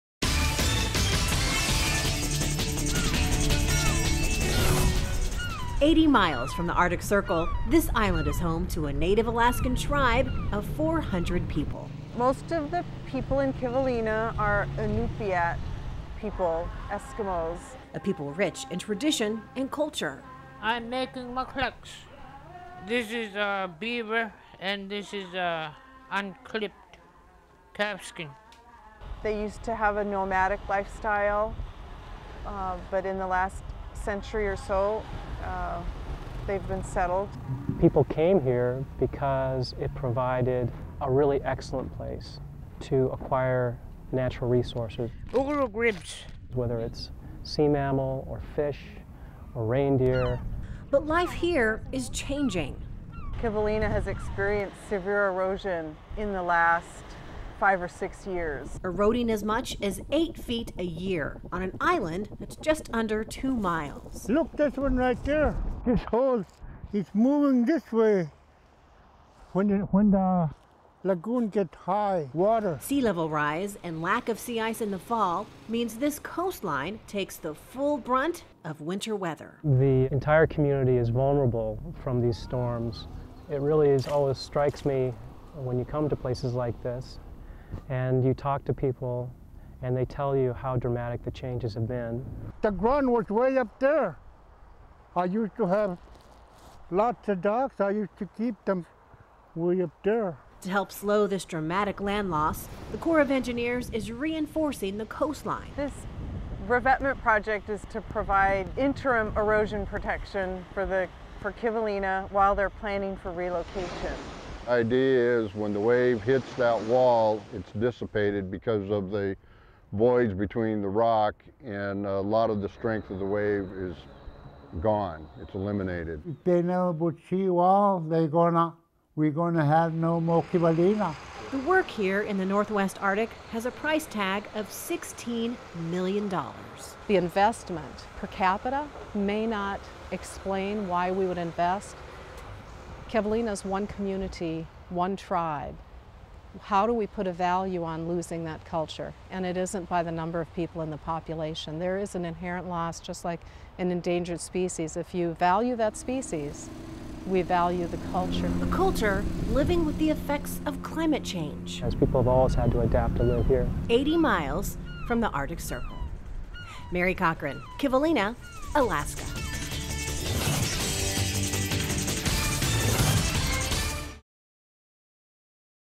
Climate change in Alaska
ARMY.MIL - Climate change is having a big impact on coastal communities in Alaska. Of the 200 Eskimo tribes that live there, 180 are experiencing erosion. The Army is helping with a 1200-foot rock revetment project managed by USACE. Mary Cochran reports from Kivalina, Alaska.